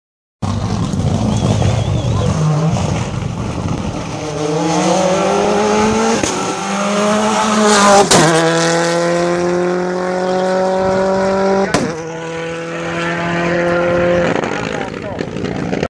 Ανάβαση Ριτσώνας 2013 Ford Escort WRC Michail Efthimiou (full HD)
Anavasi Ritsonas 16/6/2013 Michail Efthimiou me Ford Escort WRC Kirkos Racing...strofi K1 (FULL HD video)